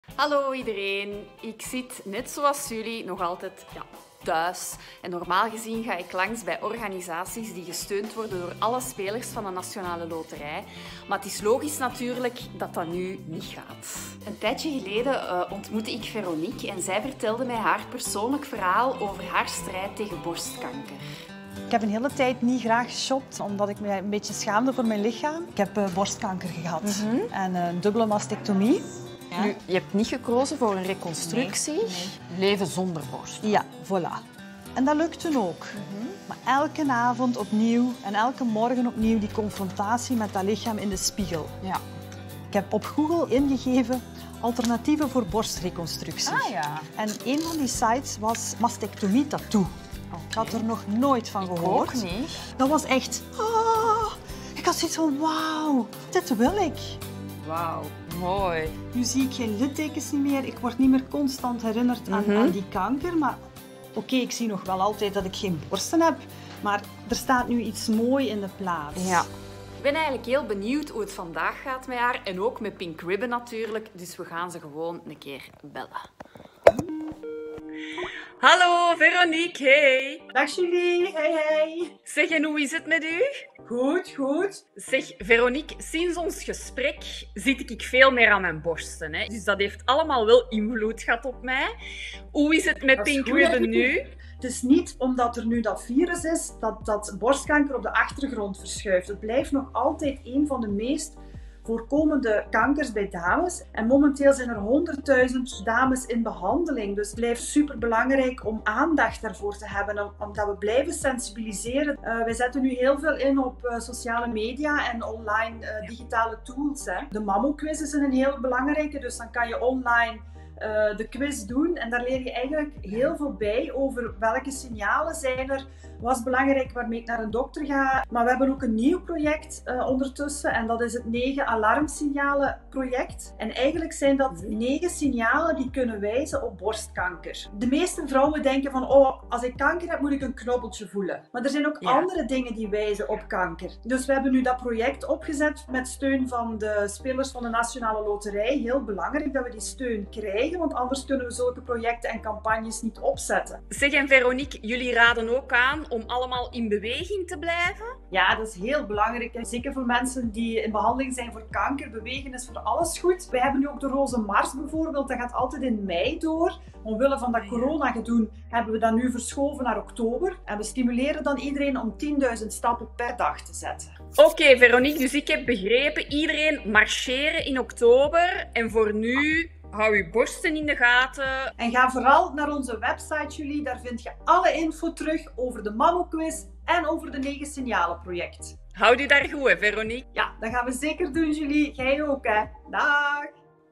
De Nationale Loterij steunt Pink Ribbon
Nu je niet zomaar naar de dokter kan zijn de digitale tools van Pink Ribbon vandaag des te effectiever. Dit zijn online sensibiliseringstools die je de kans geven om je kennis over je borsten, borstkanker en de opsporing van borstkanker te testen en uit te breiden. Die tools heeft Pink Ribbon kunnen creëren dankzij alle spelers van de Nationale Loterij